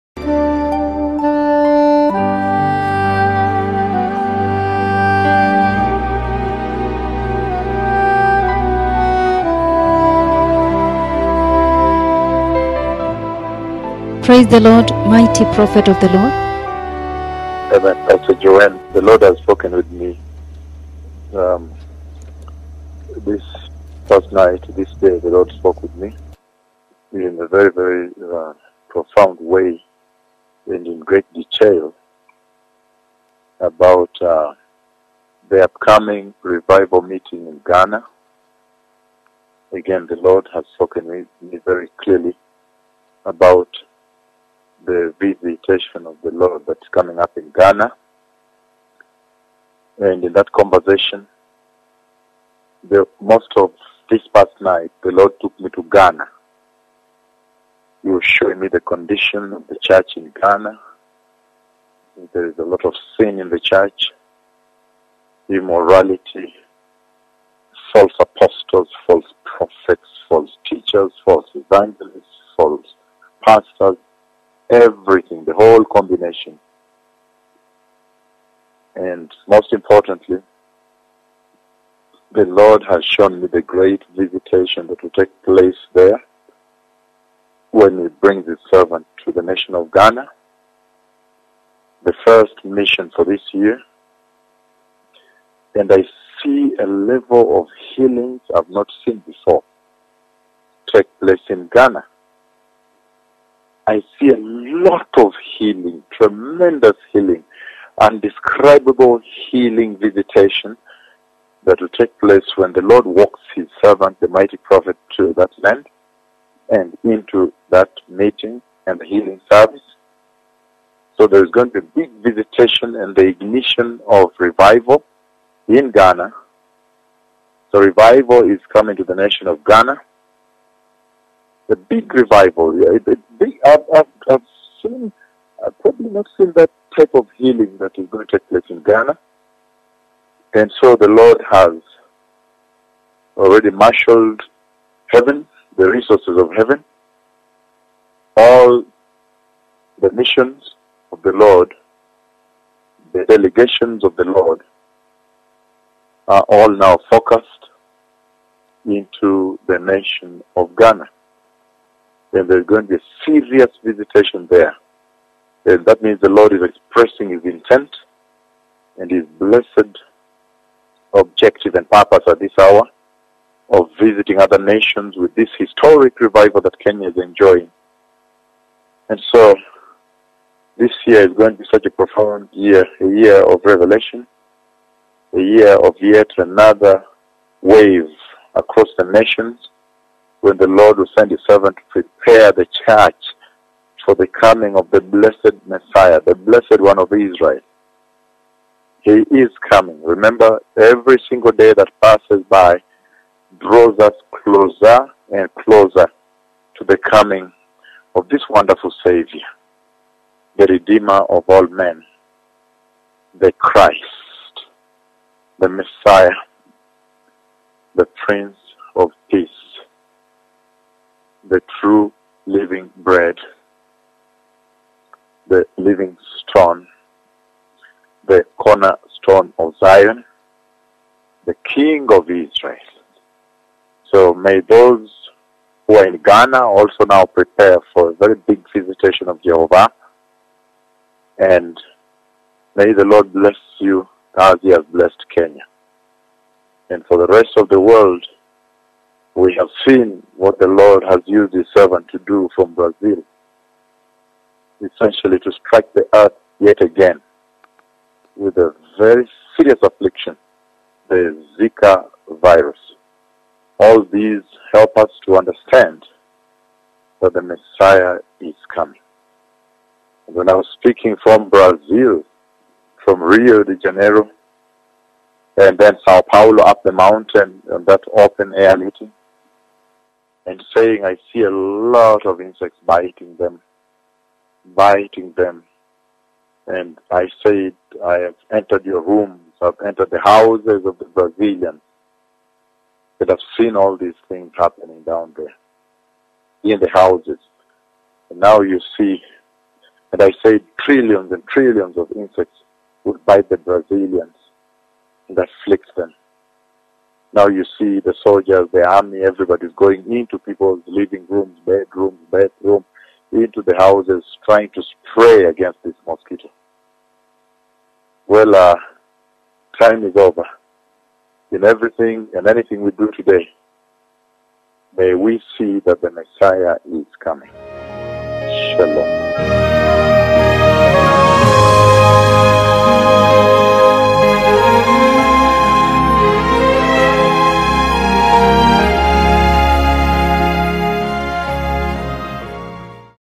PROPHET DR. OWUOR! PROPHECY OF TREMENDOUS REVIVAL COMING TO BEFALL GHANA 2016!
PROPHECY OF TREMENDOUS REVIVAL COMING TO BEFALL GHANA 2016 - PROPHET DR. OWUOR

 Let us prepare in righteousness, and holiness for the wedding supper of the Lamb is nigh!!

Shalom!